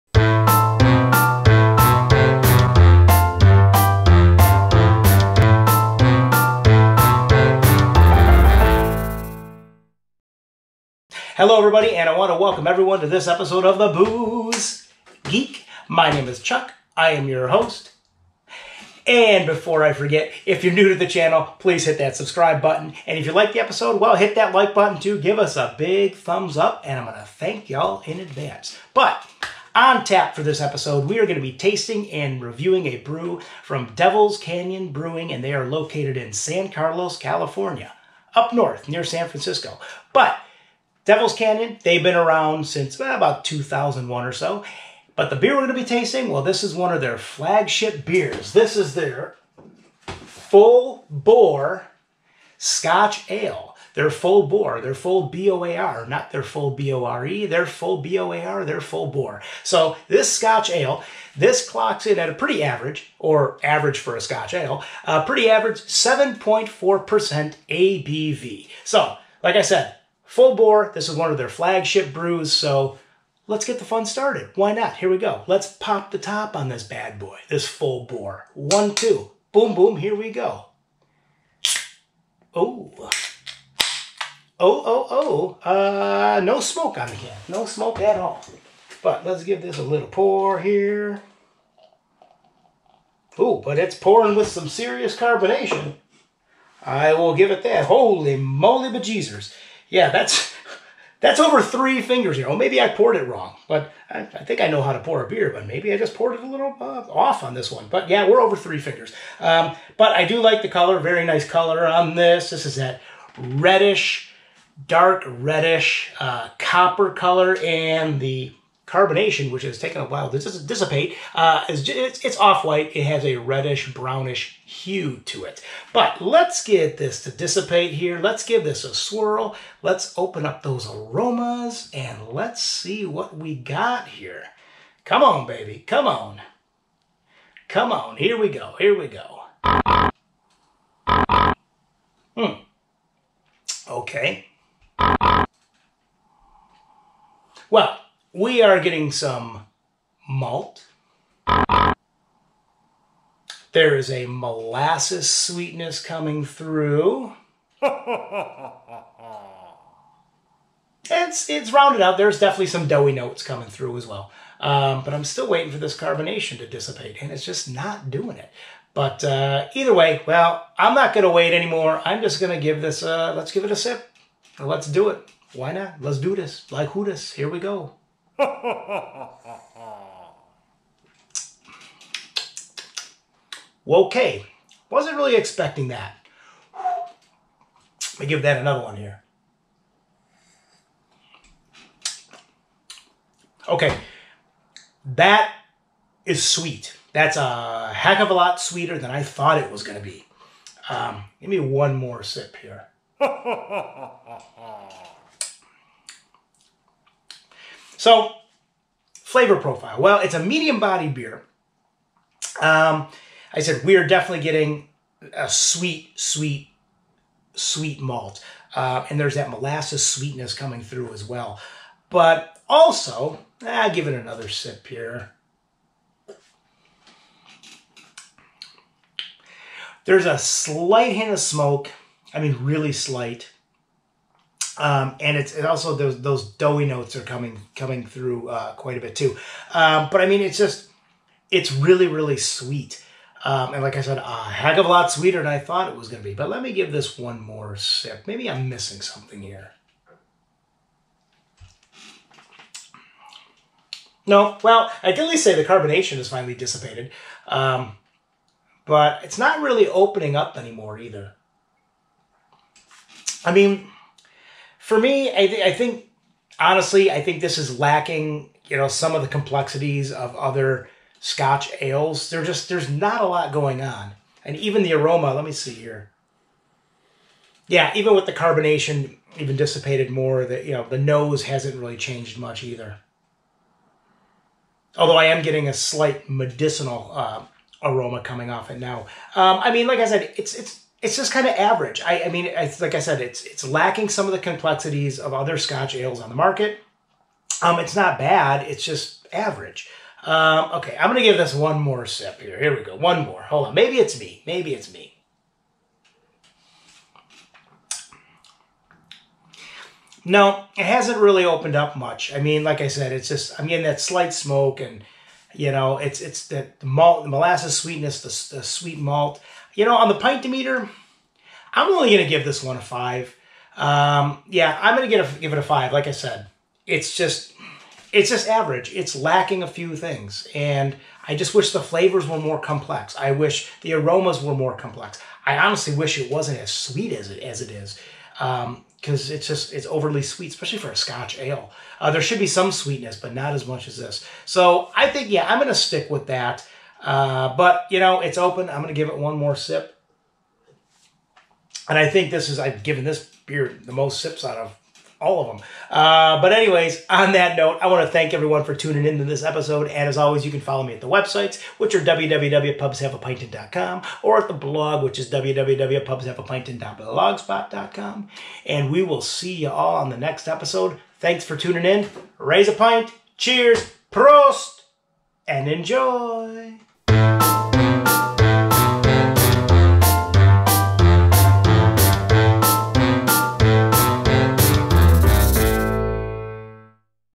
The Booze Geek - Ep. #111 - Devil's Canyon Brew Co.: Full Boar
In this episode, we taste, review, and rate, from DEVIL'S CANYON BREW CO., in San Carlos, California, their FULL BOAR. This SCOTCH ALE (and not to be confused with a SCOTTISH ALE - which is slightly different) is one of their flagship brews.

Now, first off, the SCOTCH ALES are generally heavier and fuller-bodied, with a higher ABV. While the SCOTTISH ALES are a bit lighter in body and have a lower ABV, generally around 5%. There you go! Now you do not have to look up the difference!!!

Now let's see if this FULL BOAR (and not FULL BORE) comes straight at us, head first, tusks a'blazing: And if we are lucky, maybe we can slay it!! Just like John Rambo did in FIRST BLOOD - he slayed the boar. After all, he HAD to eat something when he was stuck hiding in the cave from the cops and the National Guard. "Company leader calling Raven, come in Raven. "
Cheers, Prost, and Enjoy!!!